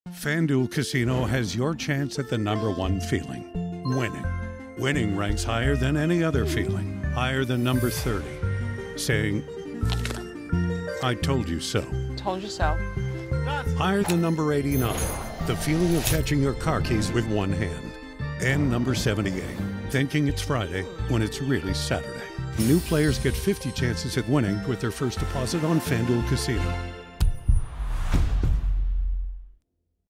FanDuel Casino “Told You So” 50 Bonus Spins
There are a lot of great feelings in the world, but nothing feels better than winning. That’s why it’s #1. Find it in spades on FanDuel Casino where new players get 50 bonus spins with their first deposit.